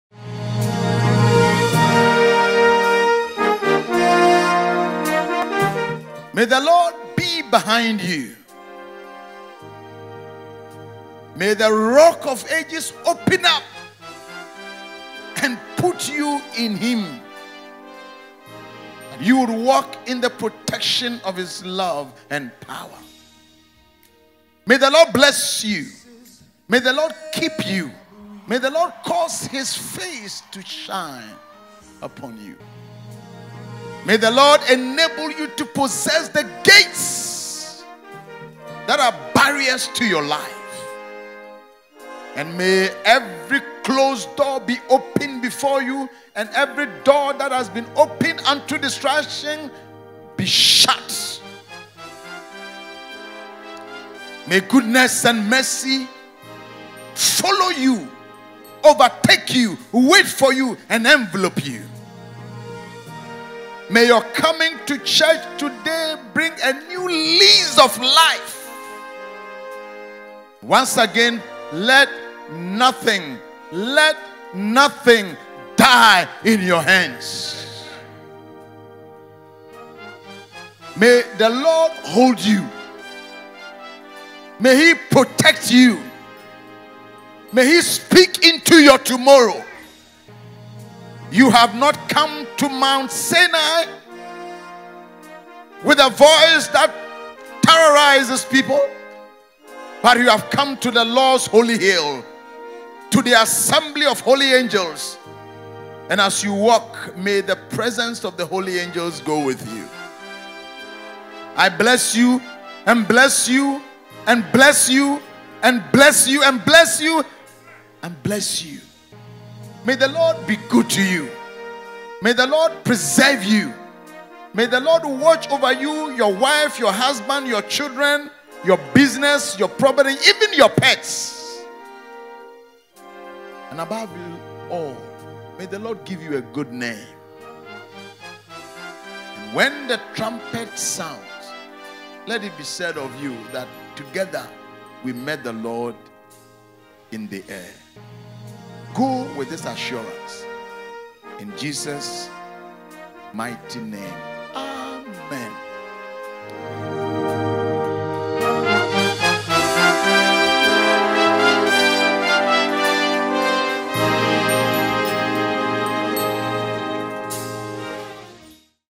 BENEDICTION By Aps. E K. Acquah. Residence Minister of PIWC Accra
BLESSED SUNDAY BENEDICTION
By Aps. E K. Acquah.
Residence Minister of PIWC Accra Kokomlemle
Ministry of the Church of Pentecost